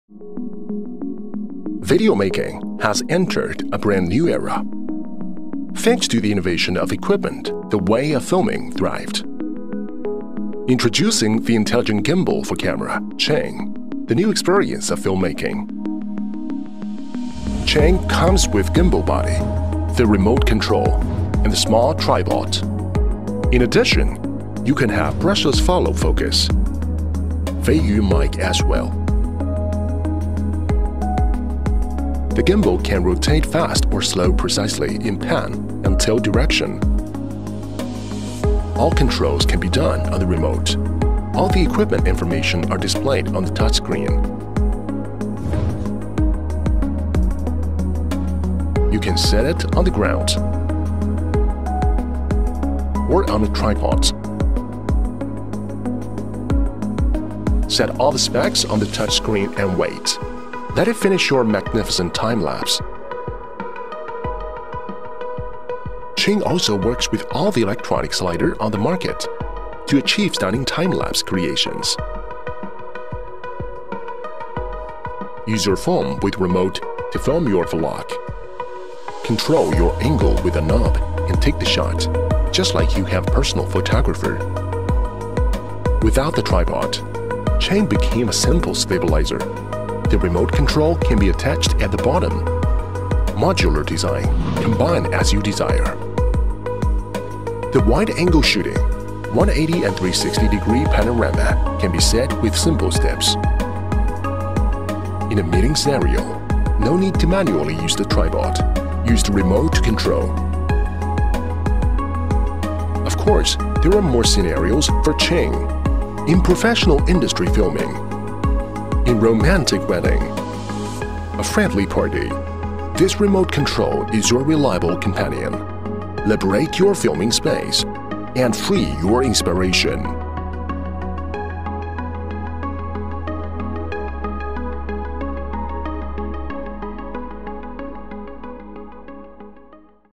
飛宇Qing智能攝影雲台重磅上線，引領延時創作新時代
Qing（擎）是飛宇科技專門針對微單、單反相機設計的智能攝影雲台，兼容市面上主流相機，可配合完成延時攝影、全景拍照、九宮格拍照等專業拍攝。